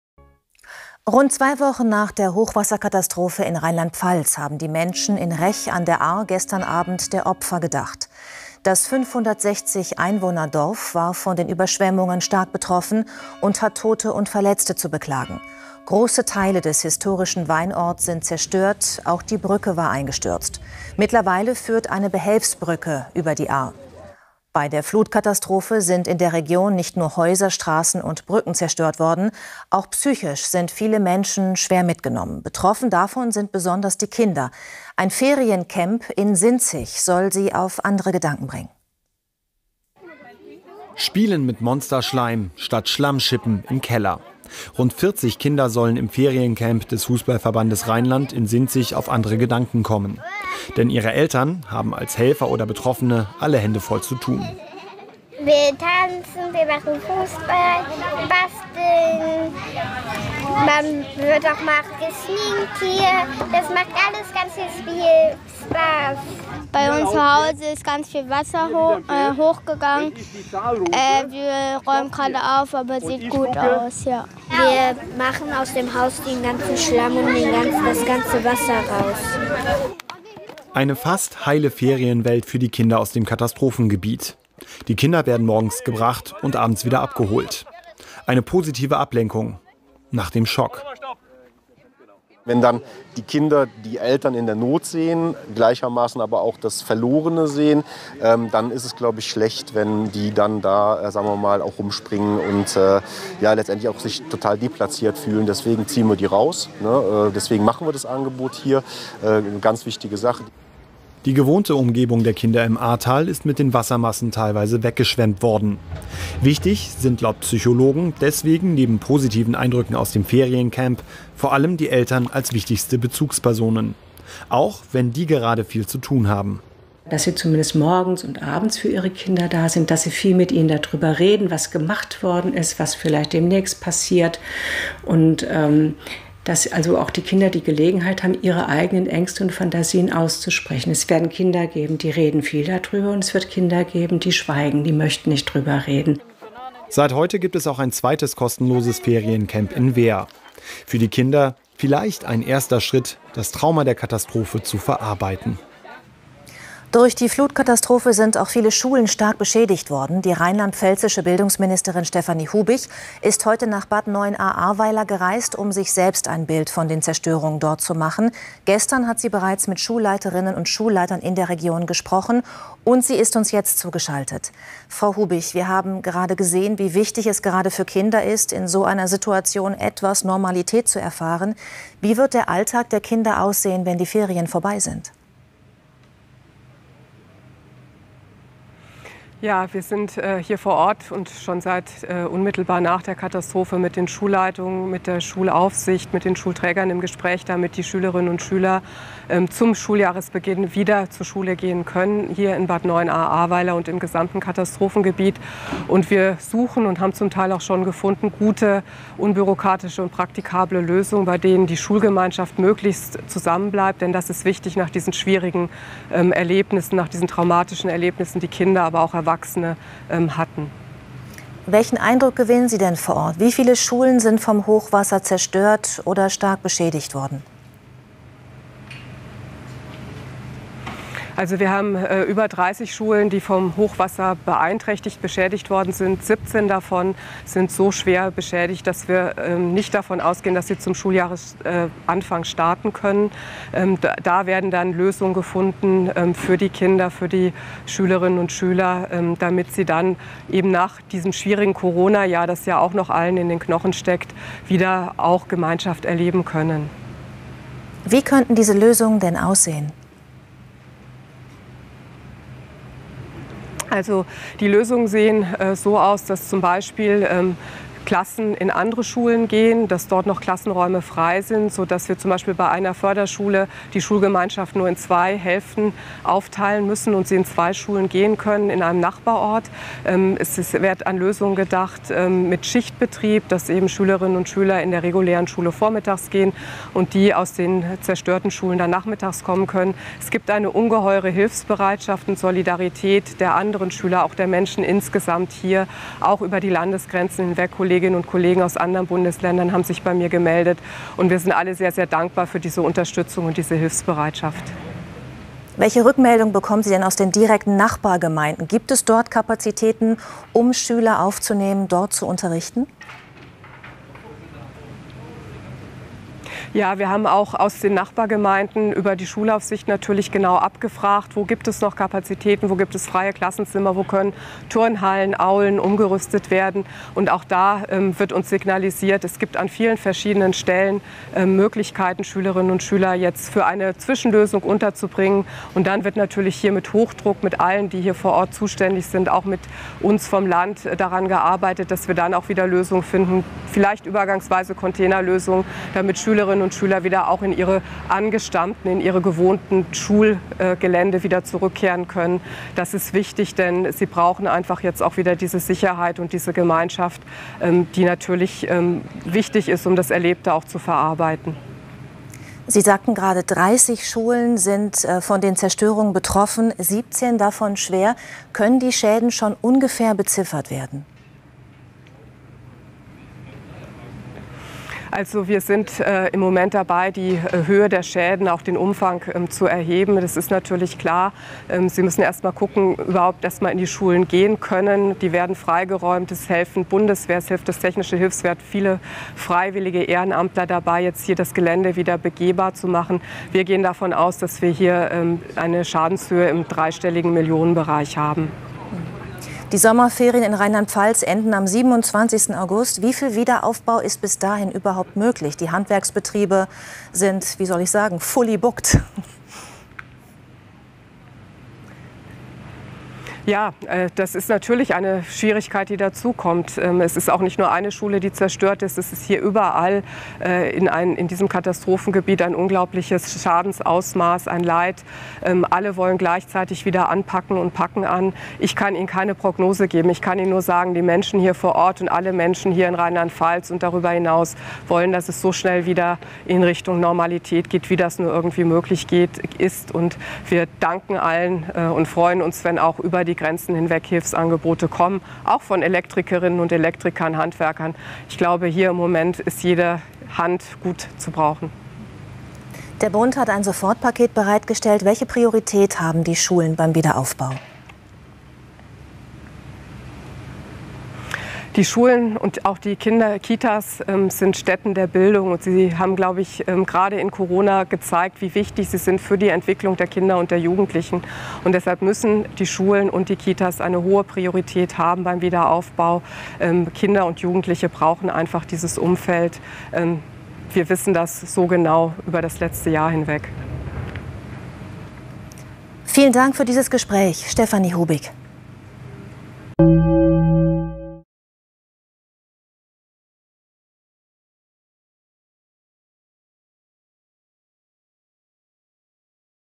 Hochwasser-Katastrophe: Gedenken in Rech an der Ahr
Für die Opfer und Betroffenen der Flut hat es einen zehnminütigen Schweigemoment im Kreis Ahrweiler in Rheinland-Pfalz gegeben. Landrat Jürgen Pföhler hatte alle Bürger:innen des Kreises gebeten, am Abend um 19:50 Uhr innezuhalten und der Opfer der Katastrophe zu gedenken. Die Kirchen wurden gebeten, den Gedenkmoment mit einem Läuten der Kirchenglocken zu begleiten.

Das Hochwasser vom 14. Juli hatte das Ahrtal besonders schlimm getroffen. Mindestens 134 Menschen starben, 73 gelten noch als vermisst.